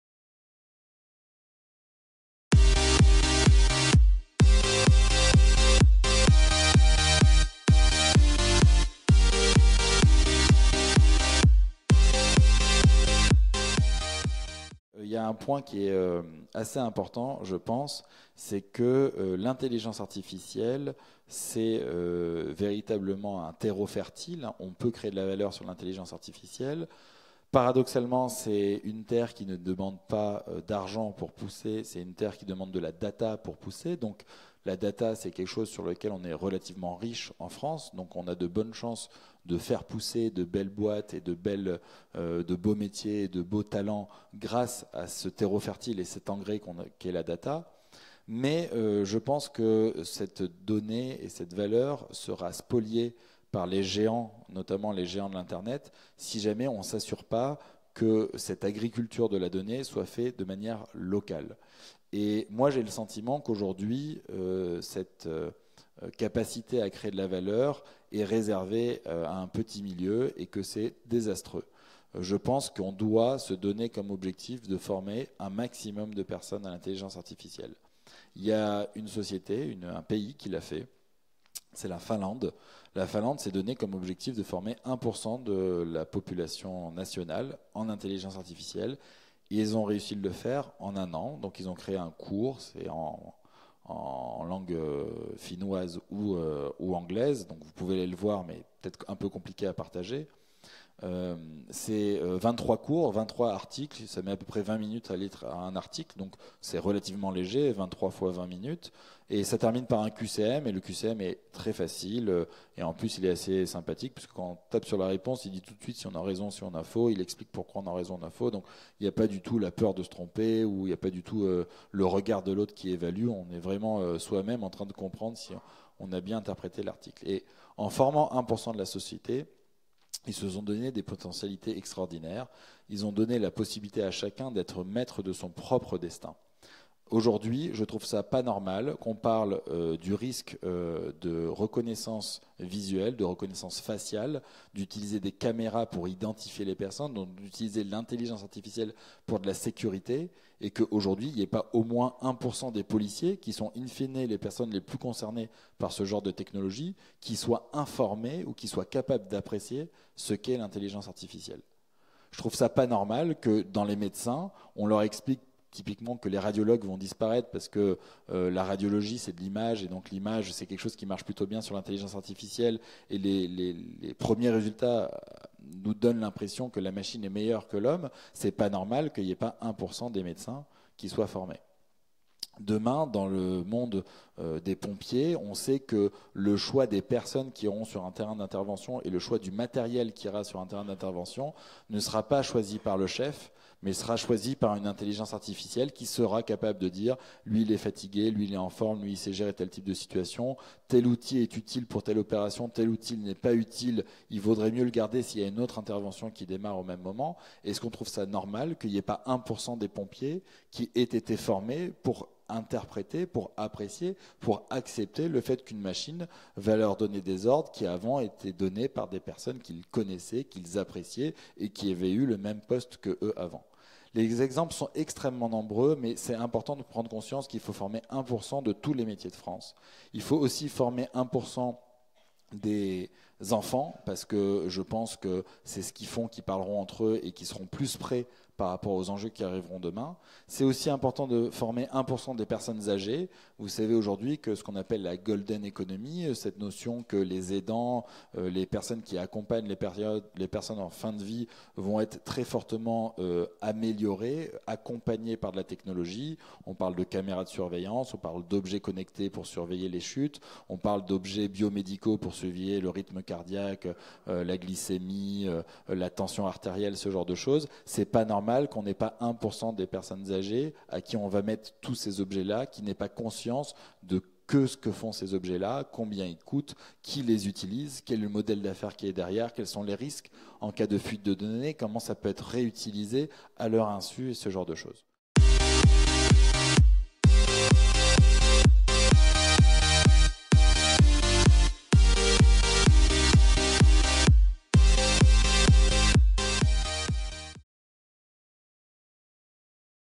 Jérémy Harroch - Former à l’intelligence artificielle toutes les strates de la société
Conférence de Jérémy Harroch, PDG et CEO de la société Quantmetry, lors de la journée mathématiques et numérique organisée le mercredi 29 janvier à Canopé Orléans.